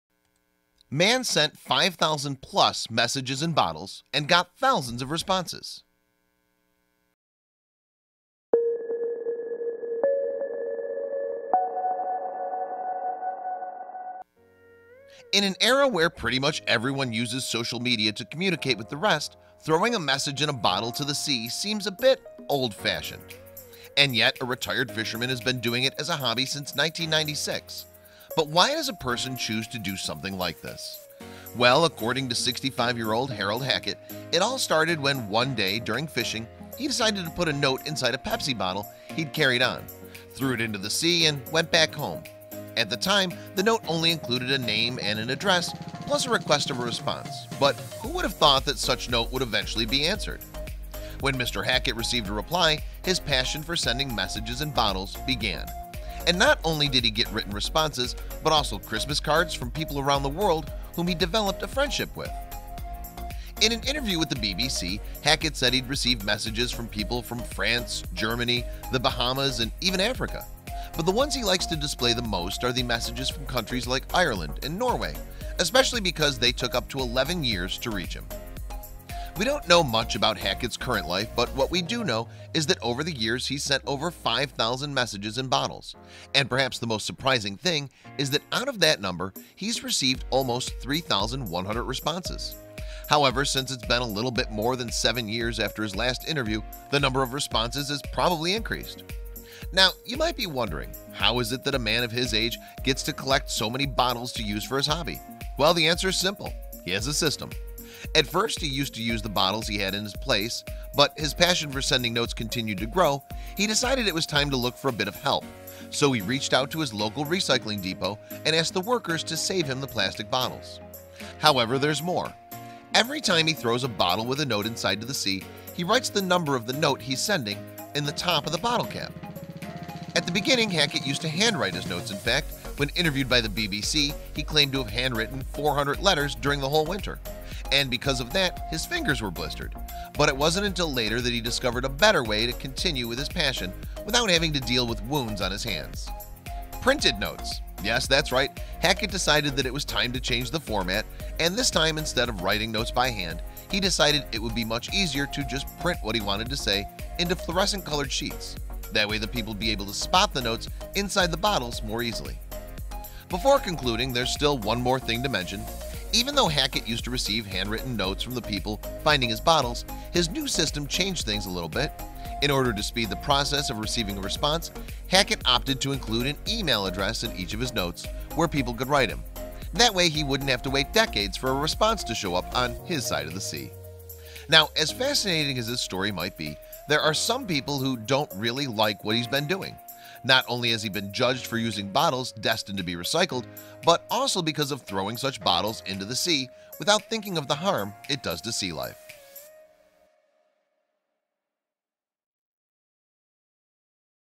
A Man Sent 5000+ Messages in Bottles and got Thousands of Responses
In an era where pretty much everyone uses social media to communicate with the rest, throwing a message in a bottle to the sea seems a bit old-fashioned.
And yet, a retired fisherman has been doing it as a hobby since 1996, but why does a person choose to do something like this? Well, according to 65-year-old Harold Hackett, it all started when one day during fishing, he decided to put a note inside a Pepsi bottle he had carried on, threw it to the sea and went back home, at the time, the note only included a name and an address plus a request of response, but who would’ve thought that such note would eventually be answered?